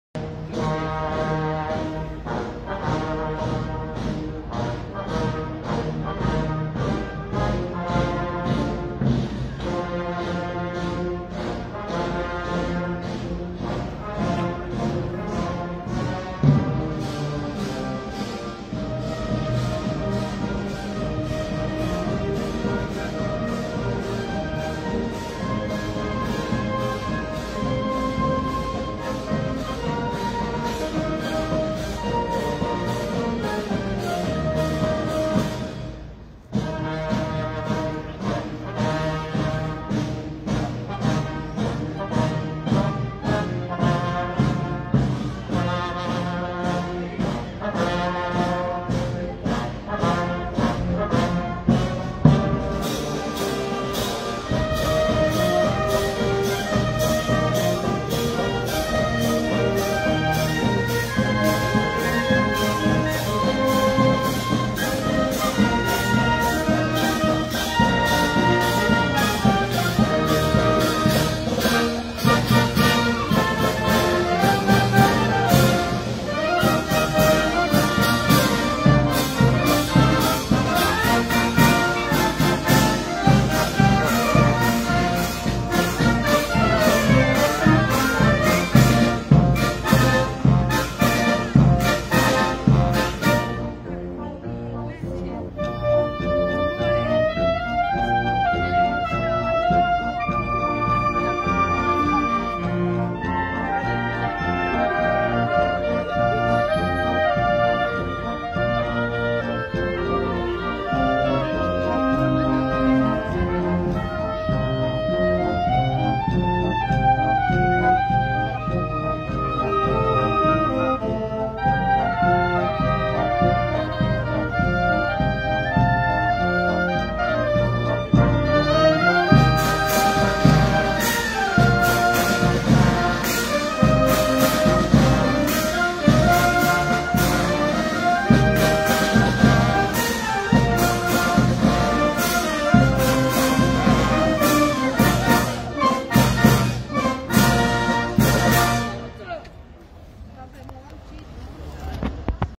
Concerto bandistico “Nicola Cassano”, diretto dal maestro Gennaro Sibilano - Marcia
Ruvo di Puglia
6 Giugno 2021 festa del Corpus Domini.